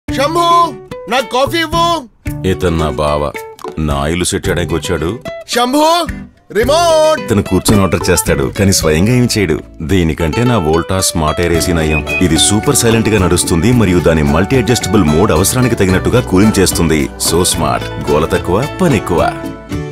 Murthy is Back - Voltas Smart Air TVC 20Sec Telugu
Introducing the all-new Voltas SmartAir AC with Super Silent Operation. Its innovative sleep mode adjusts the temperature to put you at peace while you sleep, and the 5-stage adjustable mode provides more comfort and savings. Now, live smart with an IoT-enabled interface, straight from the house of India's No. 1 AC brand.